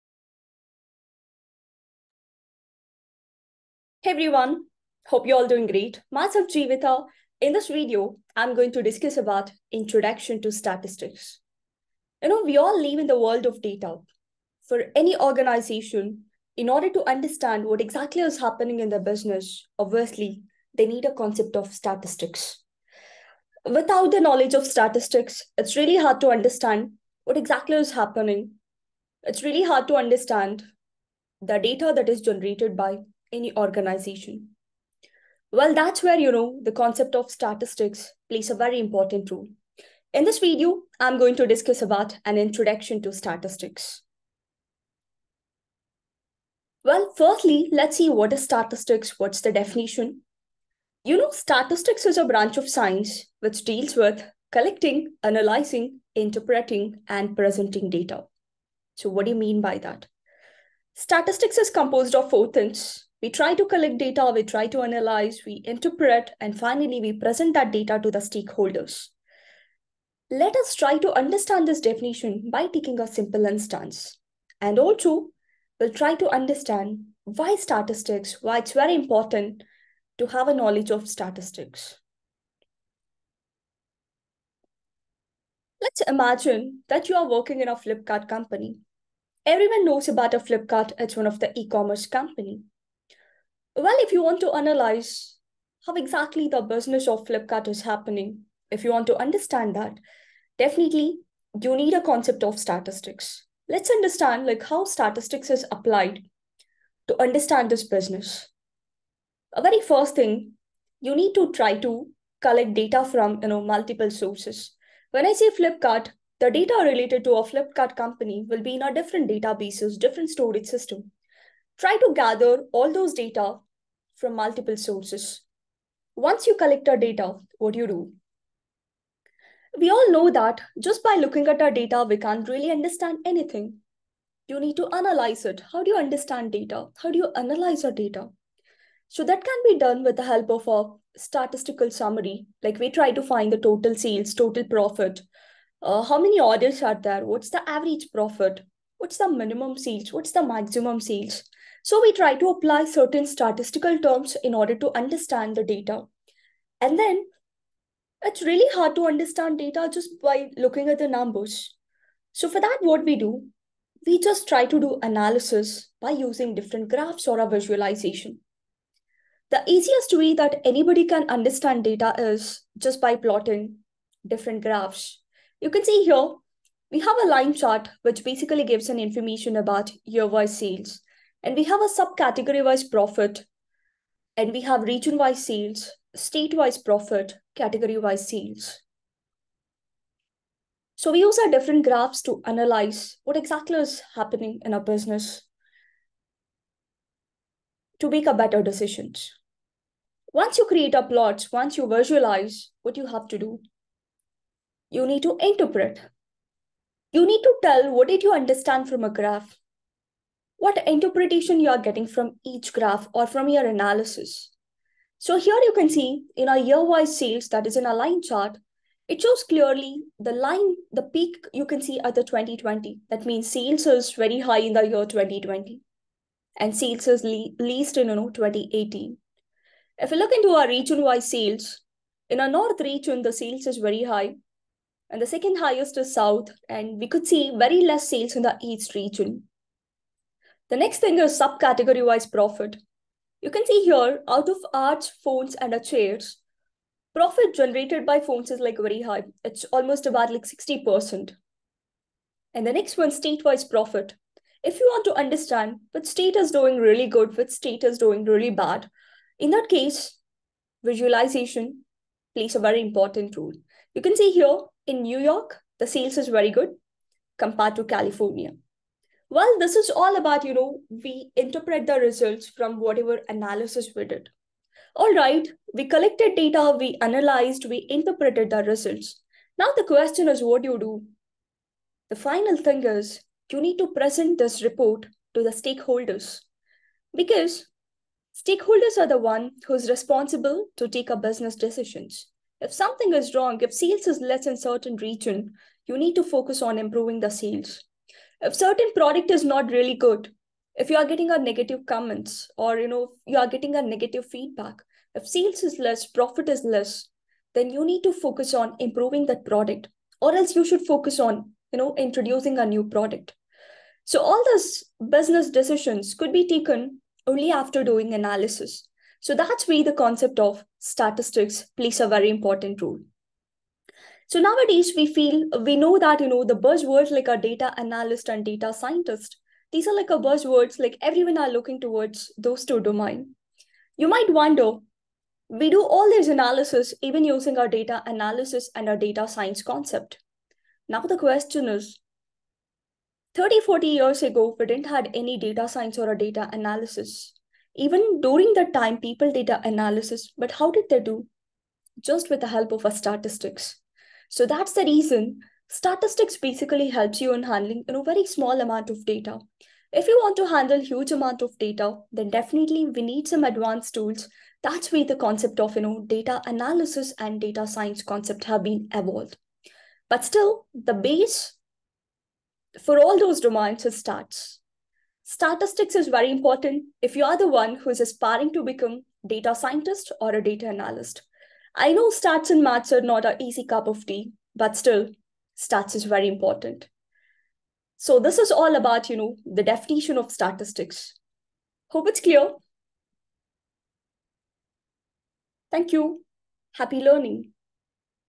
Introduction to Statistics
Statistics is the foundational pillar of data science and data analysis. It serves as the compass guiding our journey through vast data landscapes. This essential discipline enables us to systematically collect, rigorously analyze, expertly interpret, and effectively present data, providing the bedrock for data-driven decision-making. In the realm of data science and data analysis, statistics is the key that unlocks the doors to valuable insights, driving innovation, solving problems, and shaping the future.